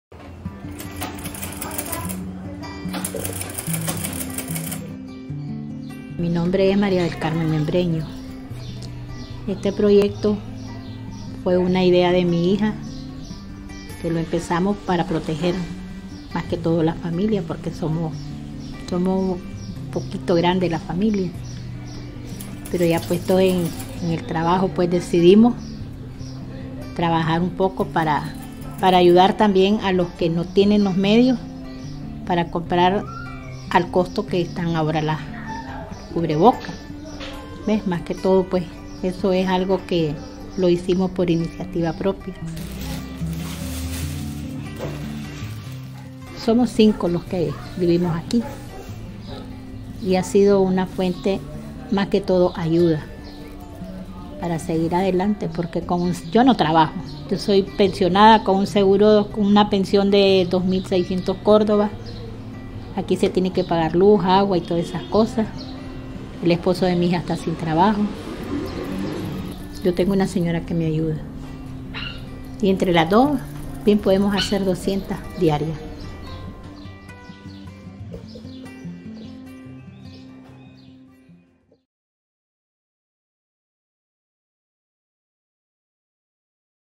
Costurera nicaragüense hace unas mil 800 mascarillas caseras semanales
La escasez de material de protección contra el coronavirus, como mascarillas y gel de manos, impulsó a la costurera nicaragüense María del Carmen Membreño, de 67 años, a elaborar mascarillas caseras, primero para su familia, y luego para venderlas en su comunidad a un precio asequible.

¡Visítenos!